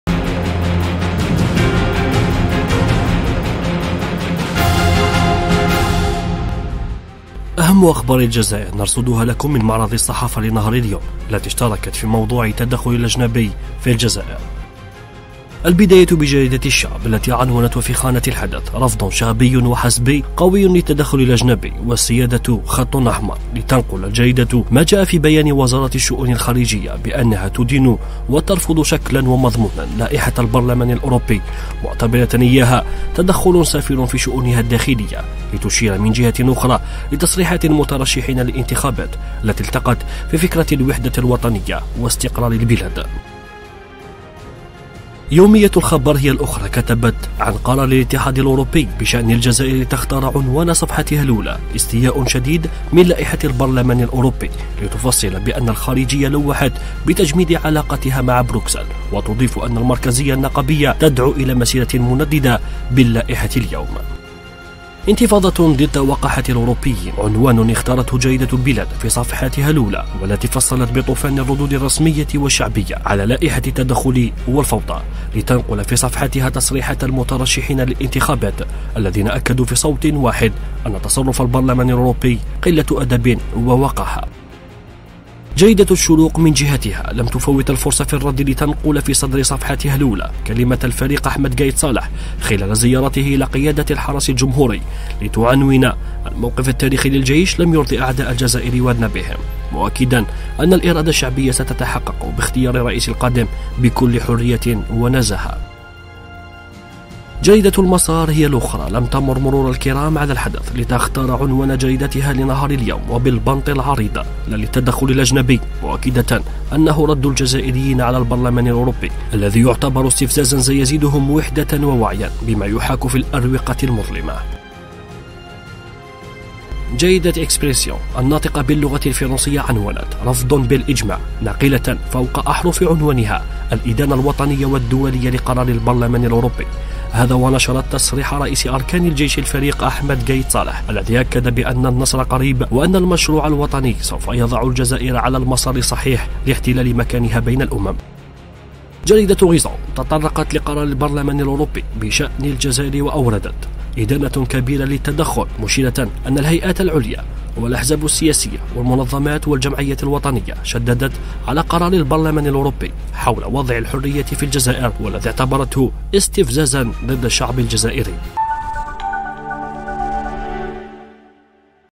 معرض صحافة يوم 30-11-2019
الجزائر (وأج)- رصد لأبرز ما جاءت به بعض الصحف الصادرة يوم 30/11/2019.